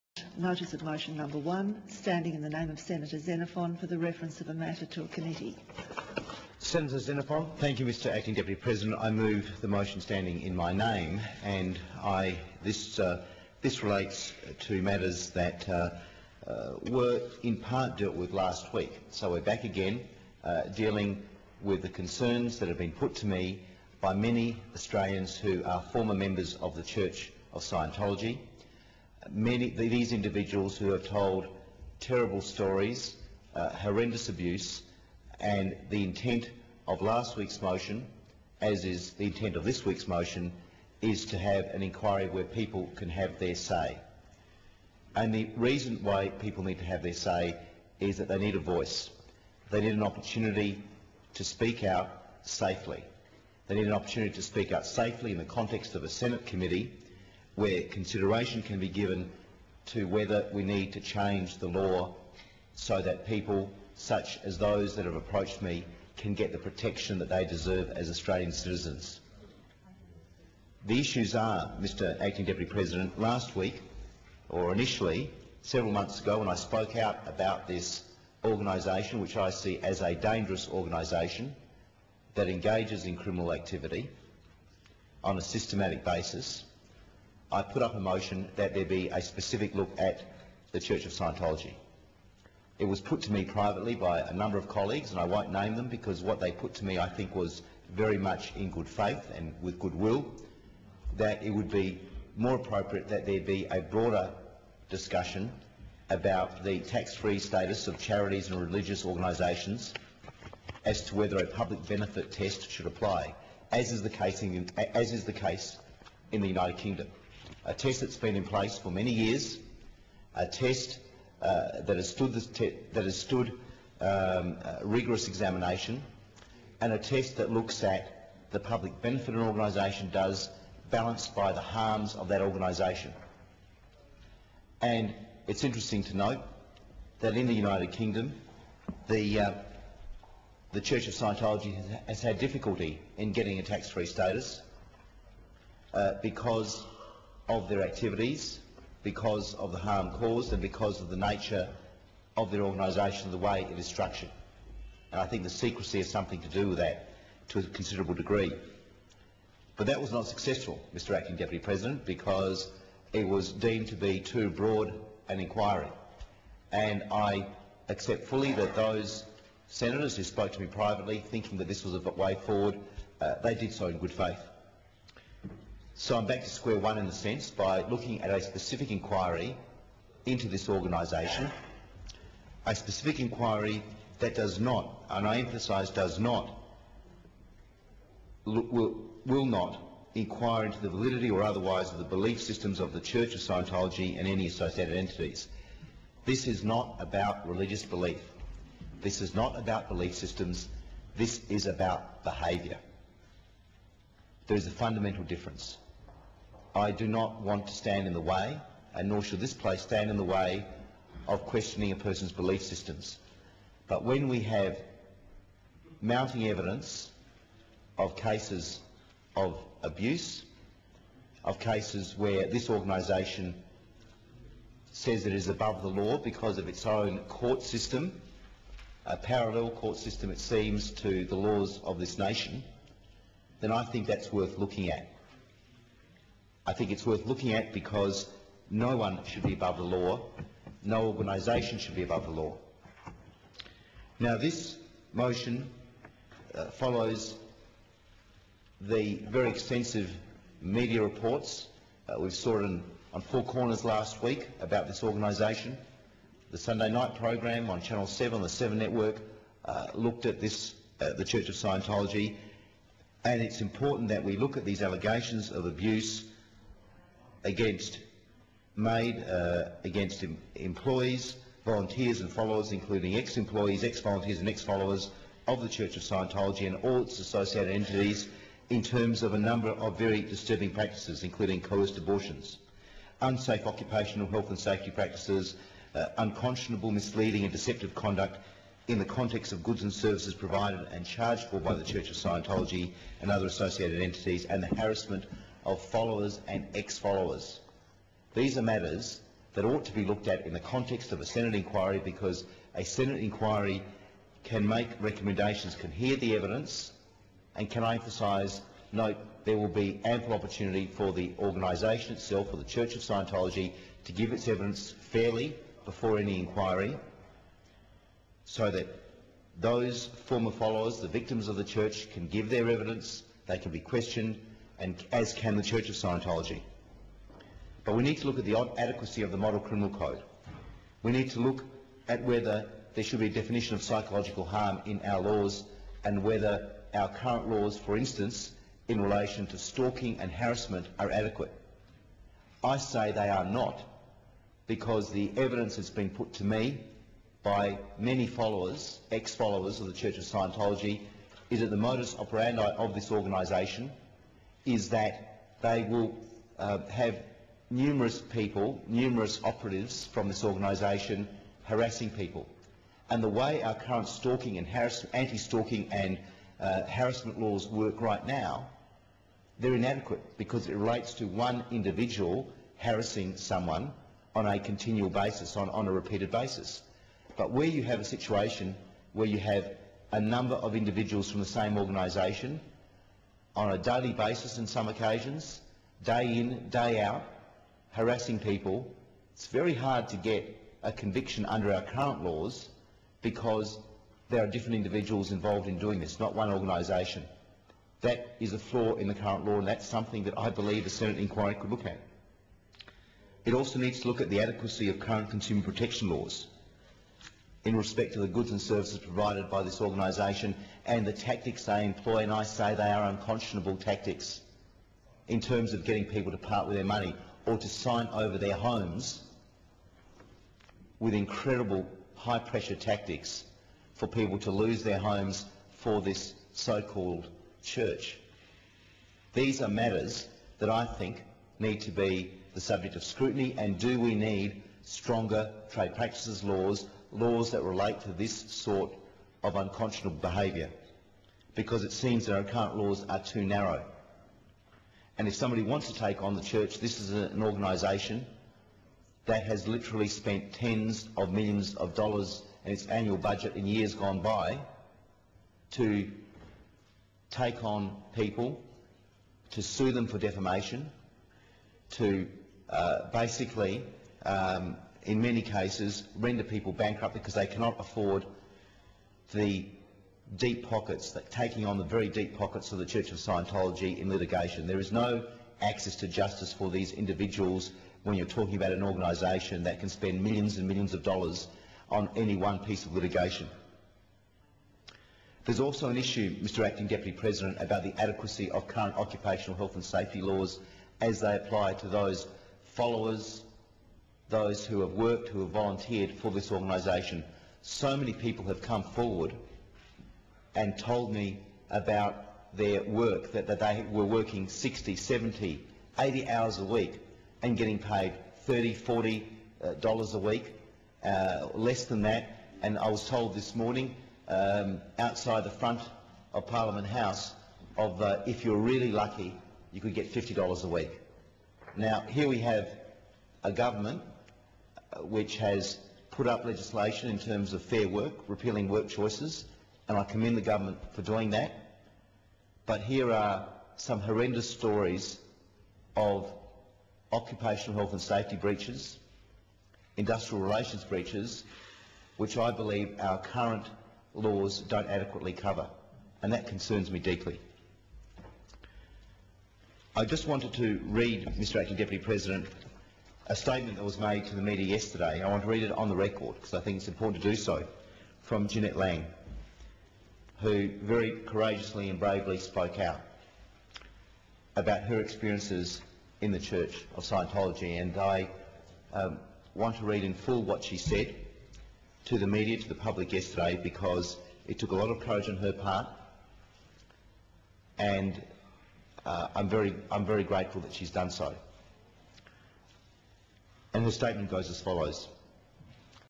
Scientology Inquiry Debate in Australia (1/10) March 18, 2010
Scientology Inquiry Debate in Australia

Senator Nick Xenophon

For a transcript, see Hansard:
aph.gov.au/Hansard/senate/dailys/ds180310.pdf